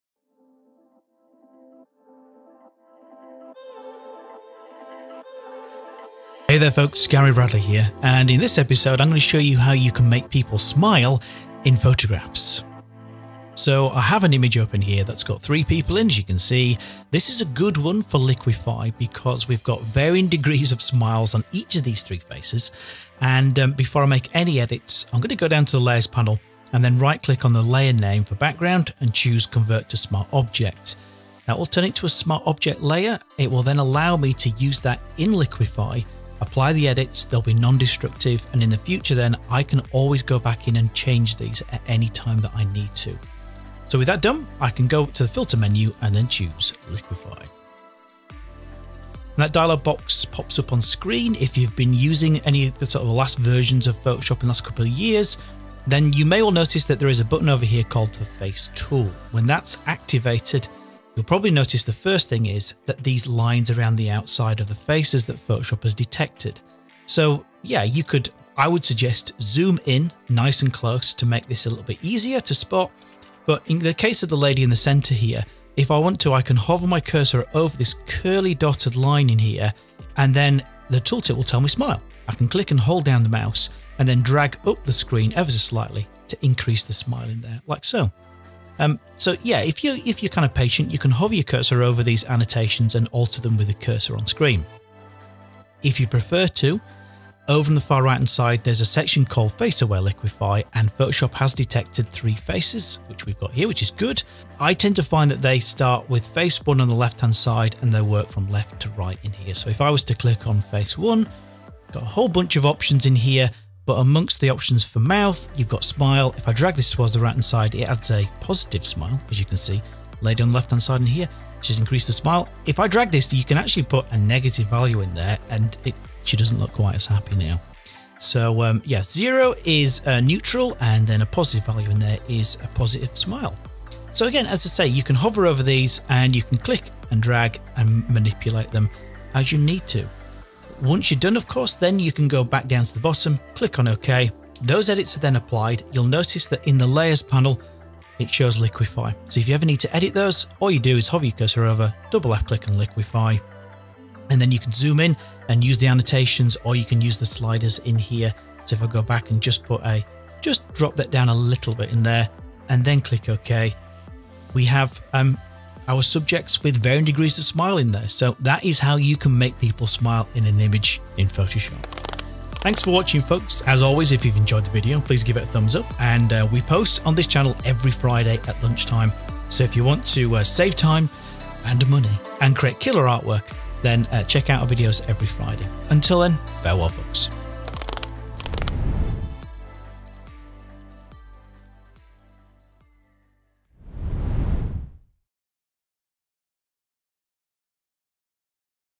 How to make People Smile in Photos | Photoshop Tutorial
In this episode, we take a look at how you can turn a frown upside down...or just make people look a little happier in photos using Adobe Photoshop. Normally this is time-consuming but with the help of Face-Aware liquify tools it need only take a minute.